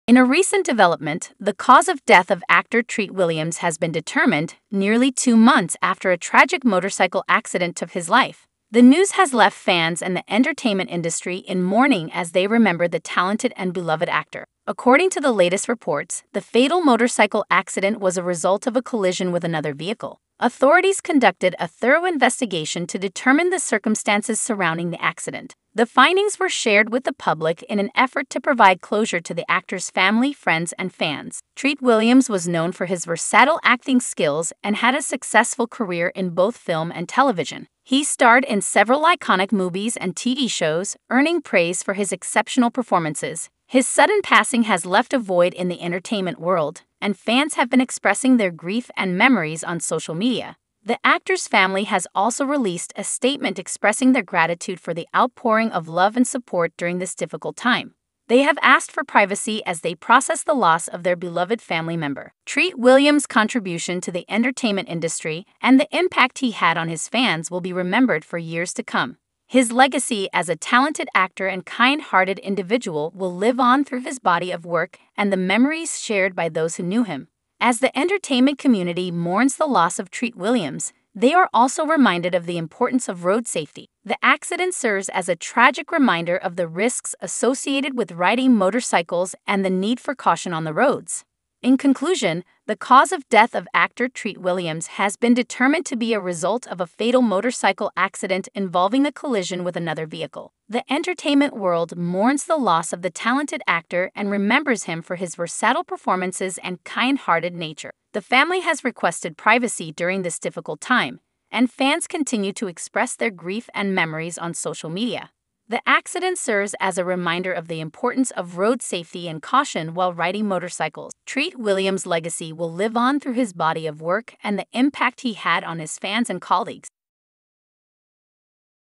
Treat Williams' Cause of Death Determined Nearly 2 Months After Fatal Motorcycle Accident
Treat Williams' Cause of Death Revealed: Determined Nearly 2 Months After Fatal Motorcycle Accident

The cause of veteran actor Treat Williams' tragic death has been determined, following a comprehensive investigation into the fatal motorcycle accident that occurred almost two months ago. The beloved actor, known for his versatile performances on stage and screen, tragically lost his life in the incident that sent shockwaves through the entertainment industry. Fans and fellow actors mourn the loss of a talented individual who left an indelible mark on the world of entertainment.